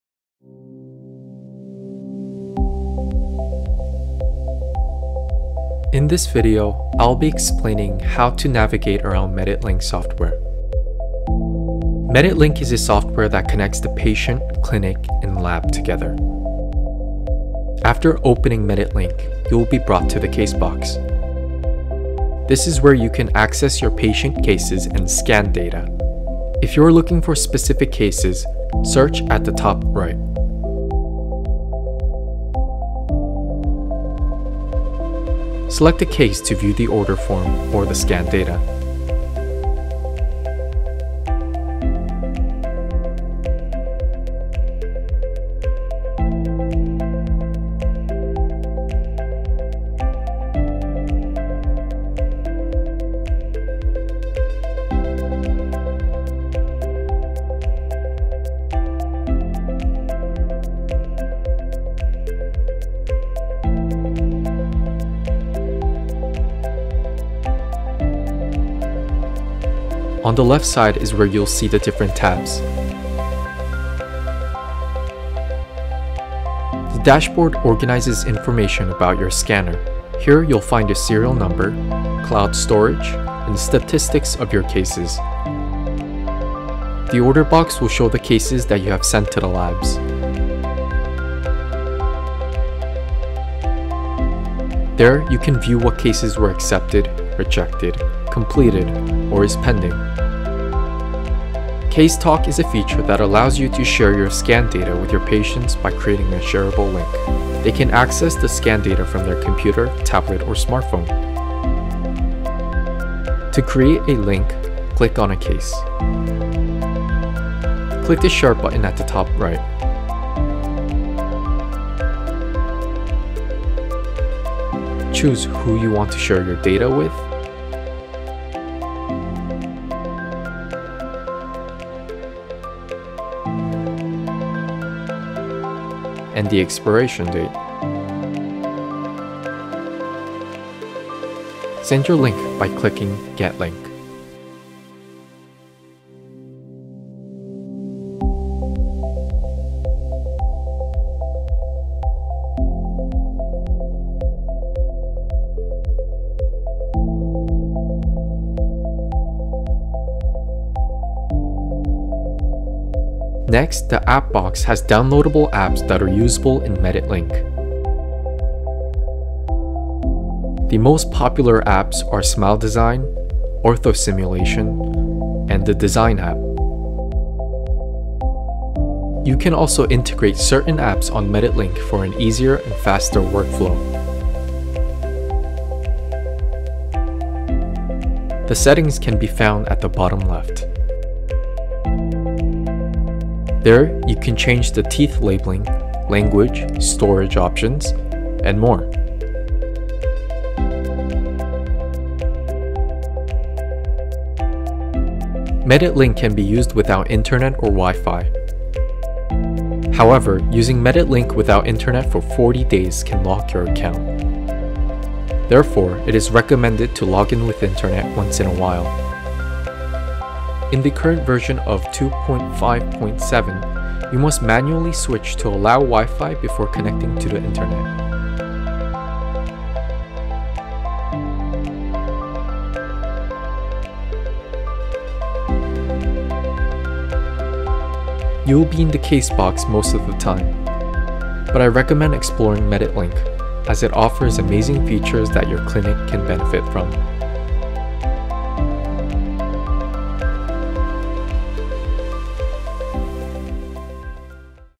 Brief Overview of Medit's Software: Medit Link
This video is a brief overview of Medit Link and how to navigate around it.

Do you need help with product information?

Do you want more information on digital products?